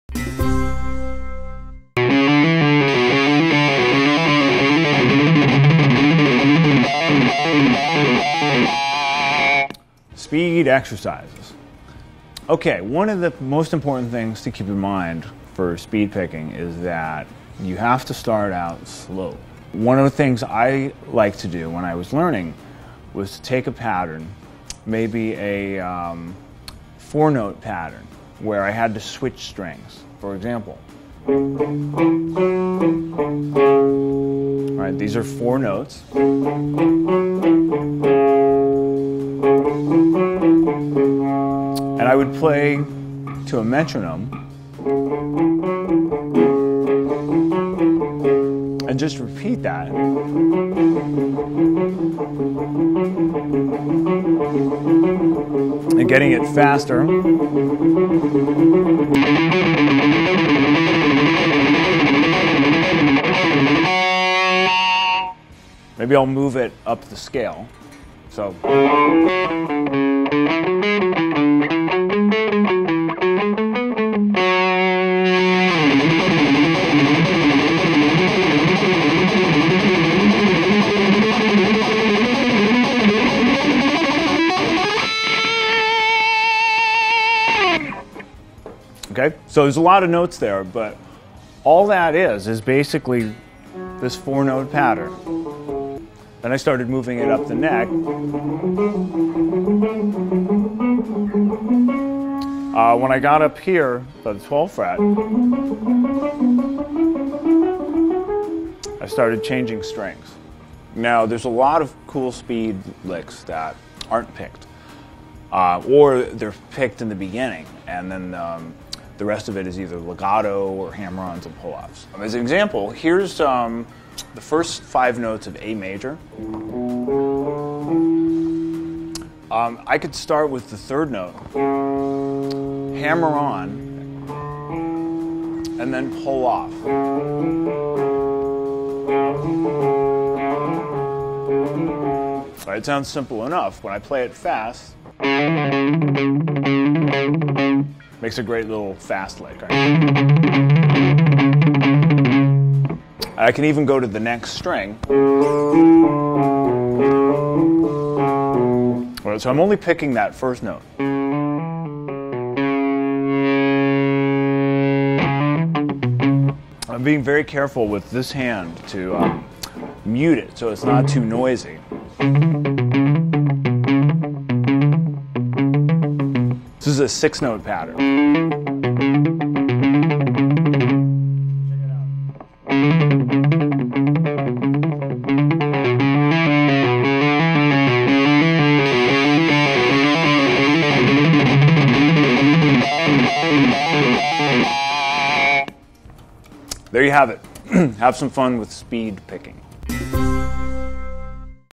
How to Improve Speed on Guitar | Heavy Metal Guitar
Speed exercises.

Okay, one of the most important things to keep in mind for speed picking is that you have to start out slow. One of the things I liked to do when I was learning was take a pattern, maybe a four note pattern where I had to switch strings, for example.

Alright, these are four notes. And I would play to a metronome, and just repeat that. And getting it faster.
Maybe I'll move it up the scale.

OK. So there's a lot of notes there, but all that is is basically this four note pattern. Then I started moving it up the neck. When I got up here by the twelve fret I started changing strings.

Now, there's a lot of cool speed licks that aren't picked. Or, they're picked in the beginning, and then the rest of it is either legato or hammer-ons and pull-offs. As an example, here's the first five notes of A major. I could start with the third note, hammer-on, and then pull-off.

It sounds simple enough. When I play it fast it makes a great little fast lick. I can even go to the next string. But as I'm only picking that first note, I'm being very careful with this hand to mute it so it's not too noisy.

This is a six note pattern.

There you have it. Have some fun with speed picking.